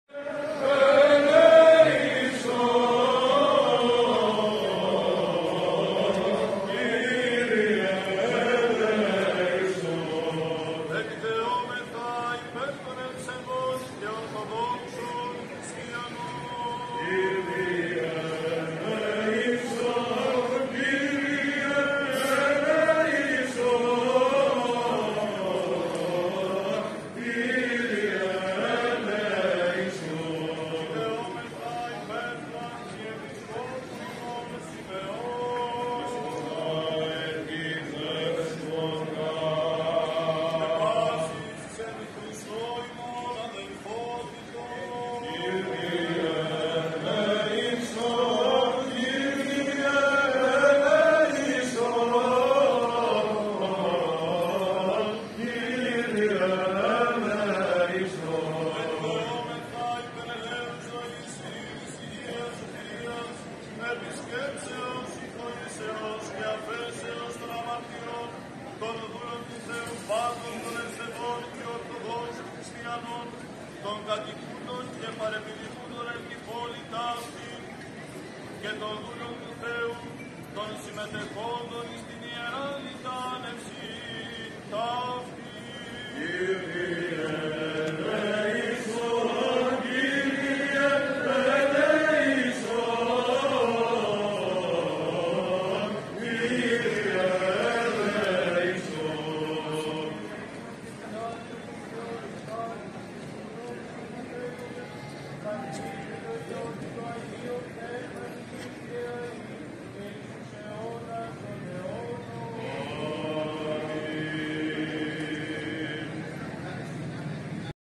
ΛΑΜΙΑ ΛΙΤΑΝΕΙΑ ΕΙΚΟΝΑΣ ΑΓΙΟΥ ΔΗΜΗΤΡΙΟΥ 26 10 2023
Η Λαμία γιόρτασε και τίμησε  την μνήμη του Αγίου Δημητρίου

Την Πέμπτη  26/10/2023  το απόγευμα πραγματοποιήθηκε με κάθε ιεροπρέπεια και λαμπρότητα Αρχιερατικός Εσπερινός χοροστατούντος του  Σεβασμιωτάτου Μητροπολίτη Φθιώτιδας κ. Συμεών και στην συνέχεια Ιερά Λιτανεία της εικόνας του Αγίου Δημητρίου.
Πλήθος κόσμου βρέθηκε στον Ιερό Ναό του Αγίου Δημητρίου στη Λαμία.